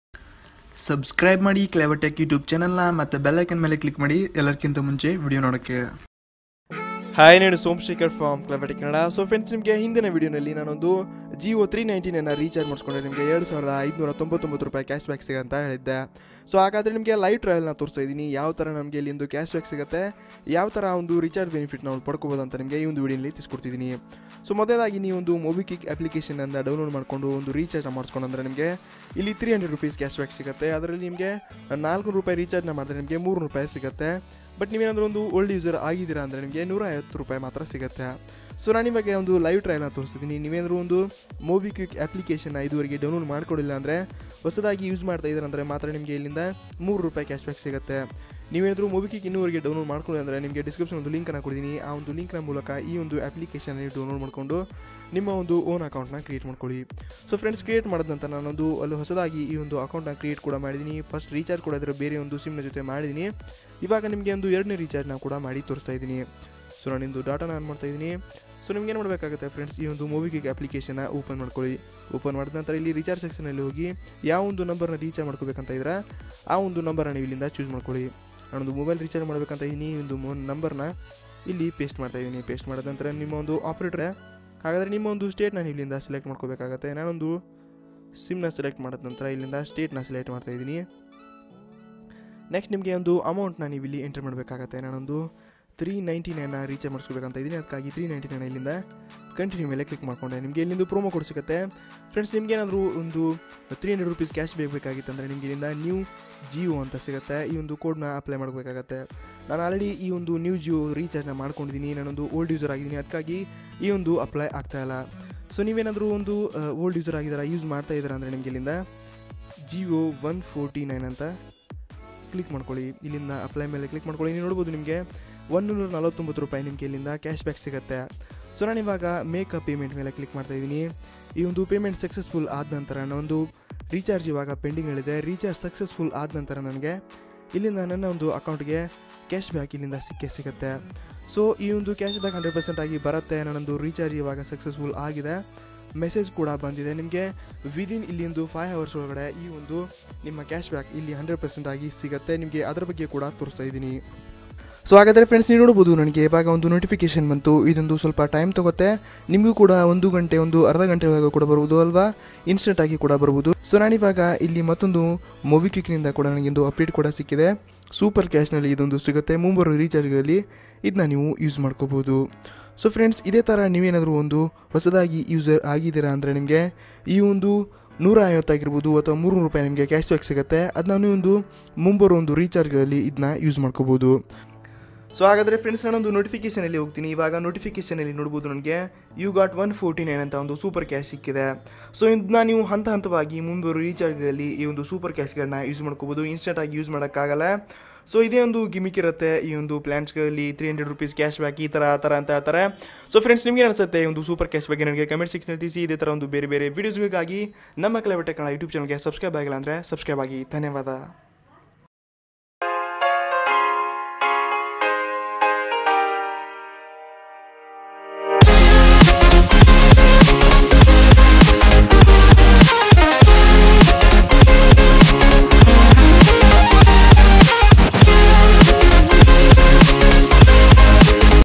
ಜಿಯೋ 399/- ರಿಚಾರ್ಜ್ ನಲ್ಲಿ 300/- Cashback | Mobikwik App ಹಿಂದಿನ ರಹಸ್ಯ! | Jio Kannada Videos
E videonalli navu jio na hosa recharge 399 madidre 2599 cashback jote Mobikwik offer ettu 300 Supercash bagge tilsikottiddivi

Refrel CODE: EAS4NR